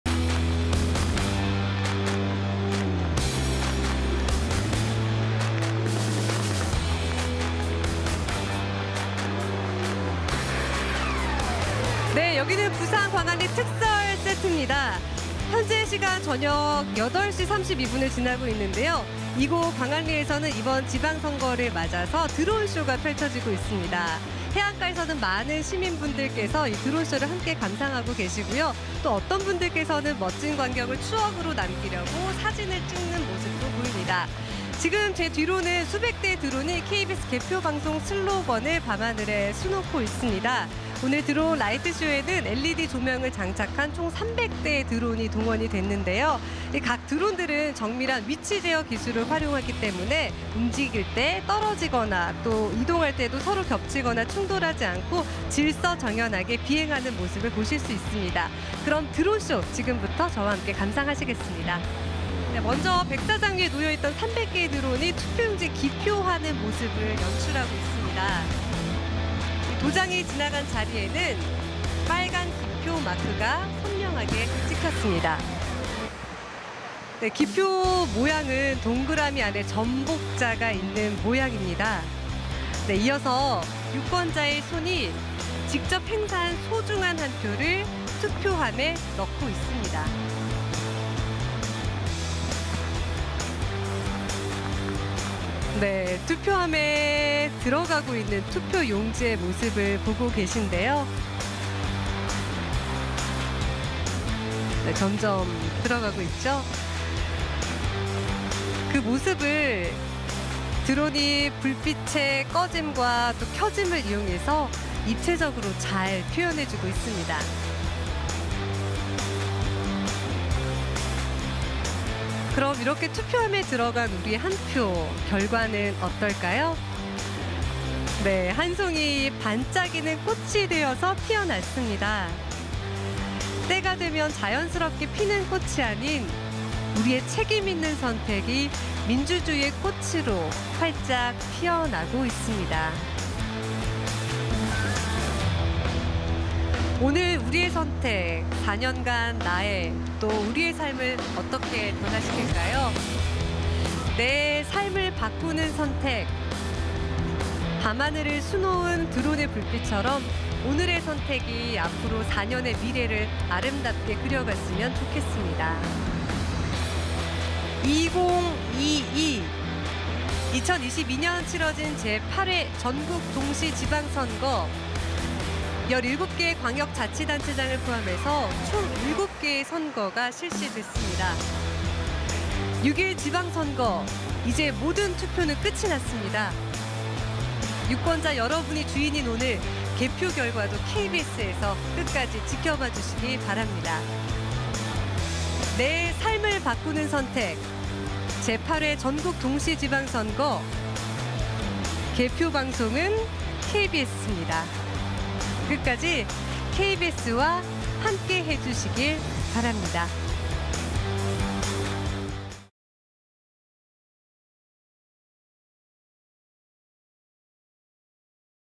[제8회 지선] 부산 하늘 물들인 `투표 드론쇼'
부산 광안대교 상공을 수백대의 드론이 수놓았습니다.

총 300대의 드론이 '투표로 밝히는 우리 부산'을 주제로 유권자가 기표하는 모습 등을 연출했는데요. 잠시 감상해보시죠~!